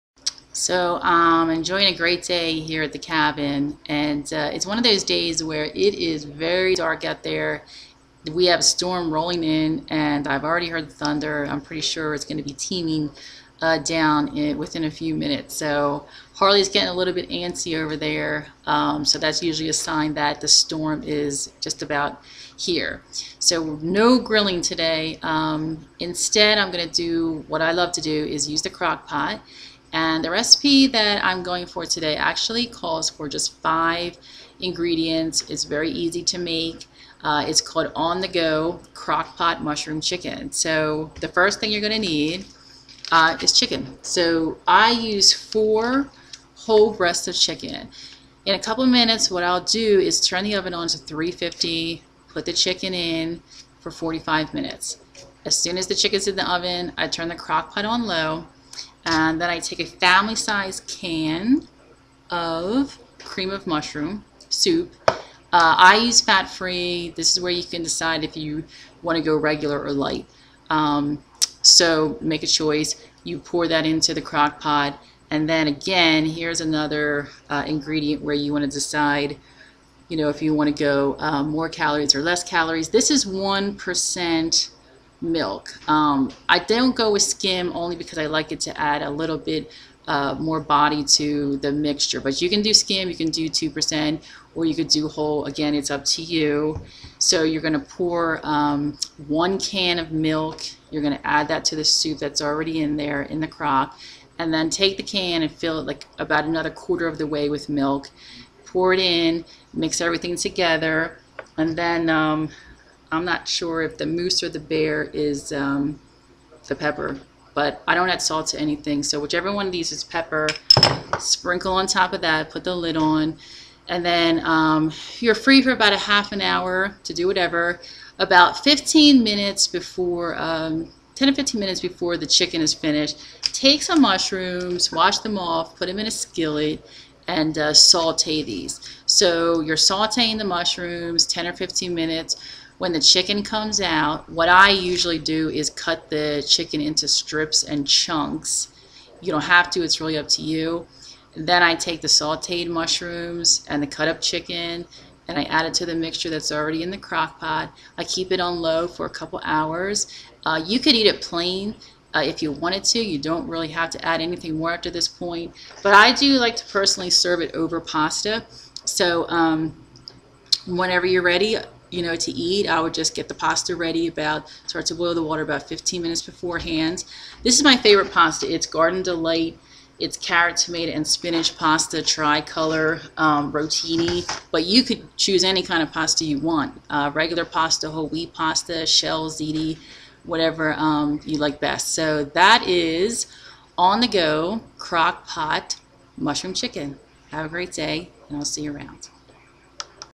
Adventures in the Kitchen: On The Go Crock Pot Mushroom Chicken
an easy 5-ingredient recipe that you can "lighten up" as you wish.